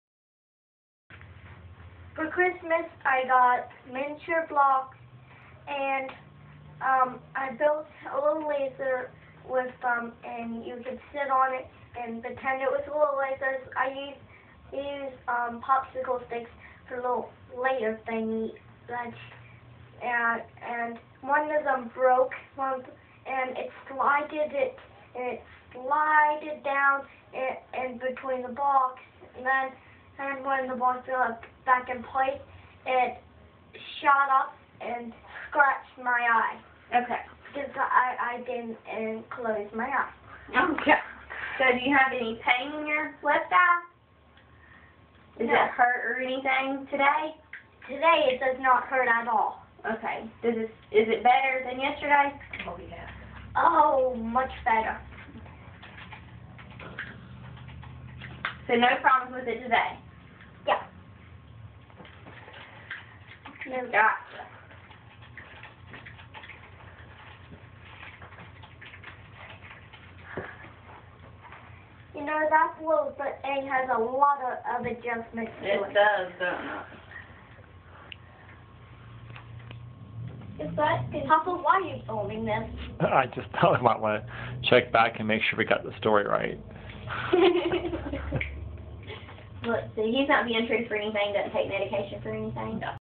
7 year old explaining how he scratched his eye.
Stratton at the eye doctor giving a very detailed explanation of how he scratched his cornea.  The doctor thought he poked himself in the eye with a stick.  Stratton's more of a thinker and inventor rather than a 'poke yourself in the eye with a stick' type of kid.